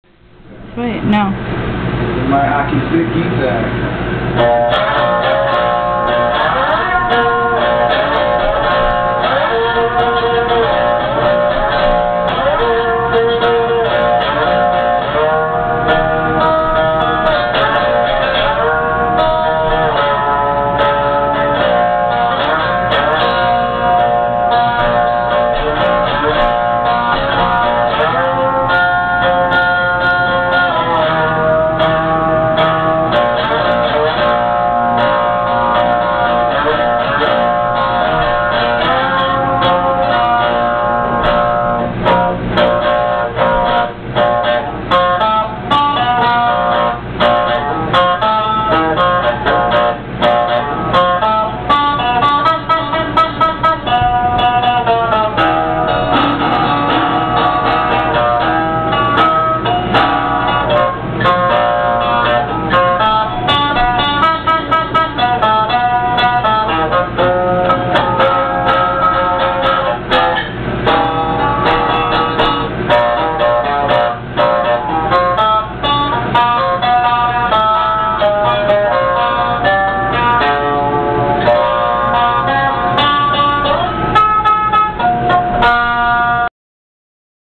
Mezzy's hockey stick guitar
This is a hockey stick that i turned into a 3 string guitar, I build alot of cigar box guitars and sell them on Ebay and elsewhere. Saw this stick at a rummage sale and thought of all the times i've seen someone pick up one and use it for an air guitar. I thought "well lets fret her up!" added some old tuners and a piezo puckup... I'm playing through a 1.5watt amp i made from a cigar box...